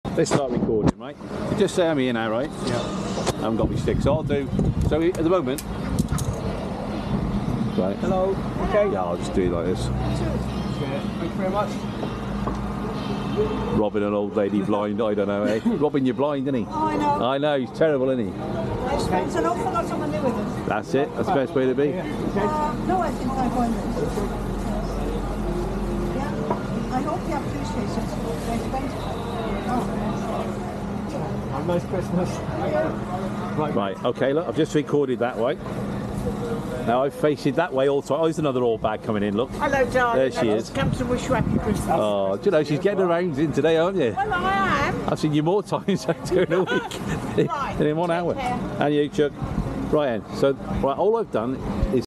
Newtown powys Wales Market Days Before Xmas 2022 short vid
Newtown powys Wales Market Days Before Xmas 2022 short vid having a laugh with the oldies
Amazing how many old bags you see around